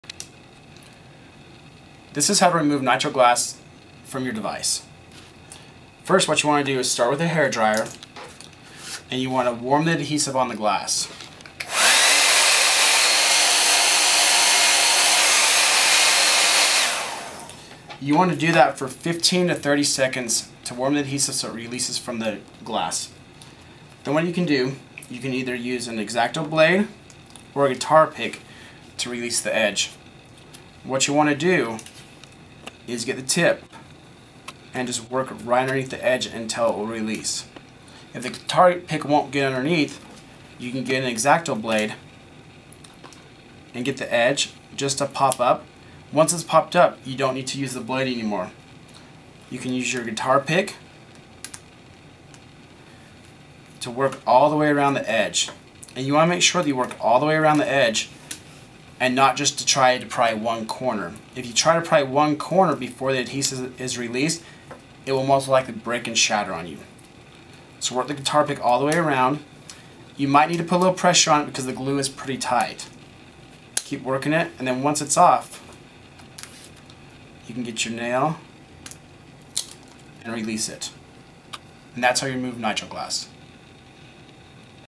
How to Remove Nitro Glass
These are some simple tips and tricks to help you in the event you should ever choose to remove your Nitro Glass protector from your device.